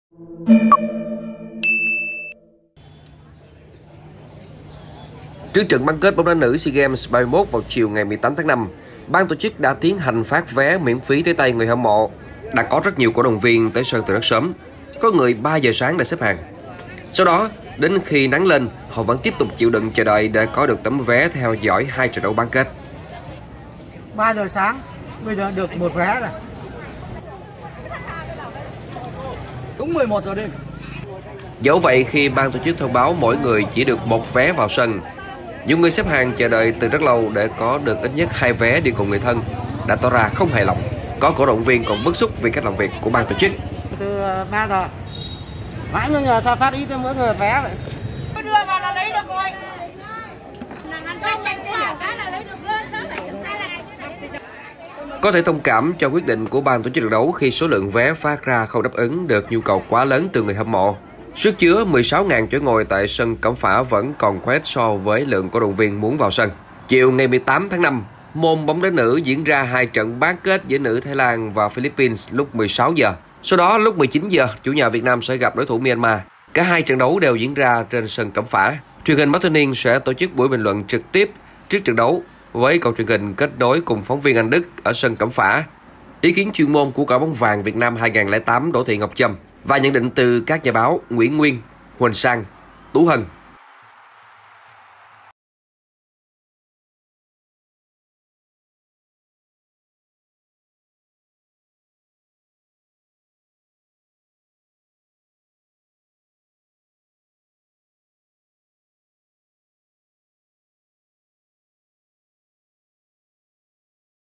CĐV xếp hàng từ 3 giờ sáng để nhận vé xem bán kết bóng đá nữ
Rất đông người hâm mộ đã có mặt và xếp hàng mua vé tại sân vận động Cẩm Phả đã chờ đón trận bán kết môn bóng đá nữ SEA Games 31 giữa đội tuyển nữ Việt Nam và đội tuyển nữ Myanmar.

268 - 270 Nguyễn Đình Chiểu, Phường Võ Thị Sáu, Q.3, TP HCM